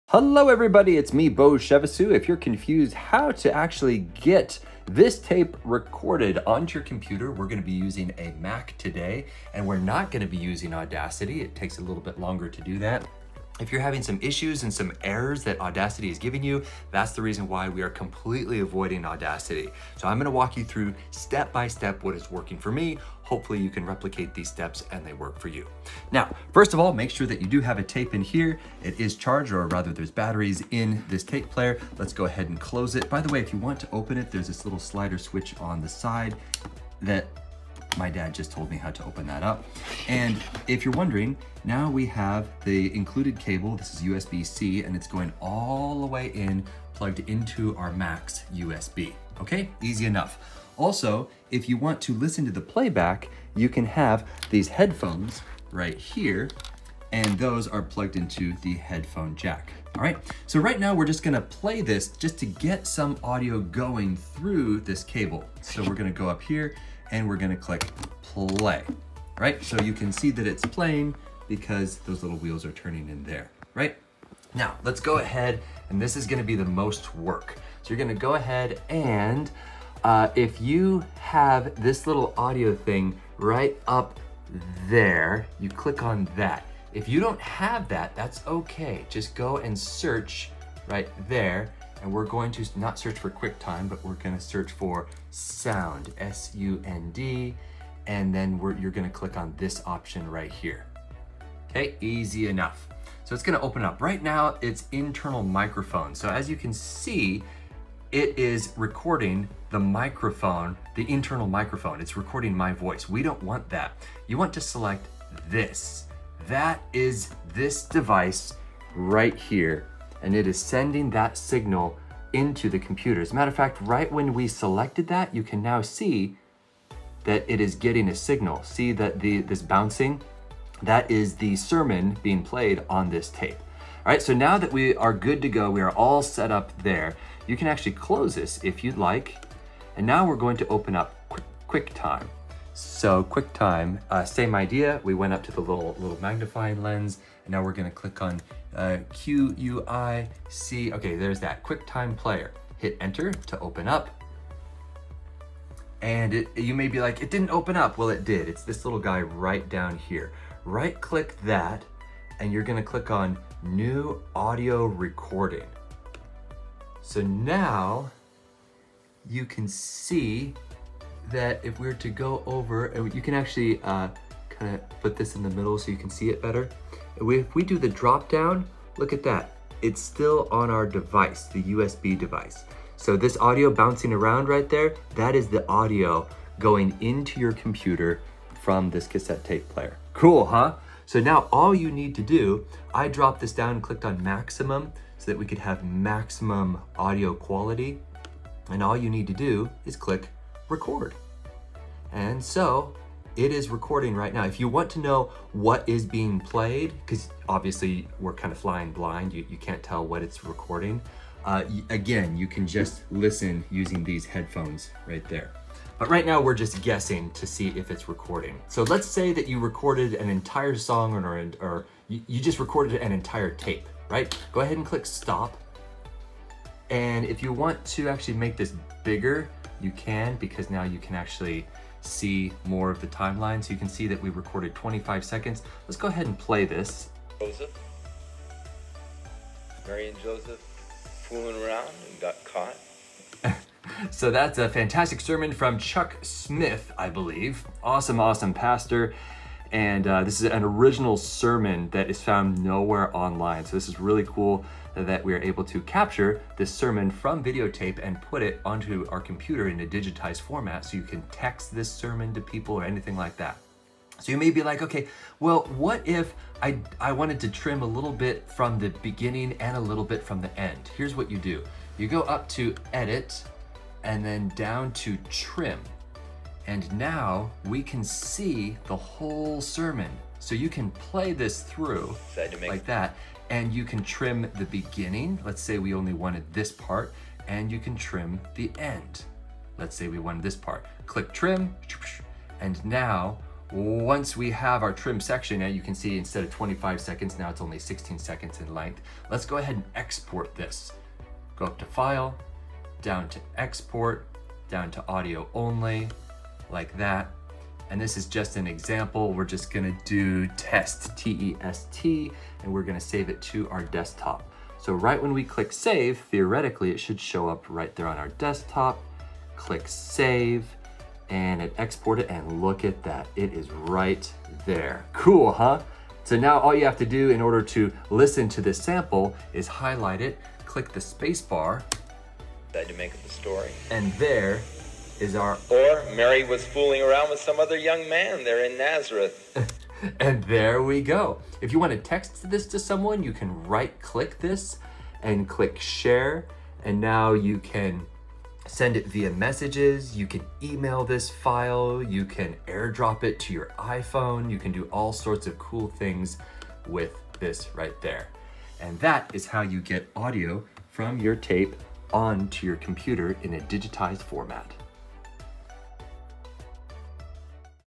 ★★★★★ Convert cassettes to MP3s - Reshow Cassette Converter - NO Audacity Errors - Mac Computer
Here’s this same tape converter on Amazon
In this tutorial I’m using a MacBook computer without audacity to convert a cassette tape sermon to a digital format. ￼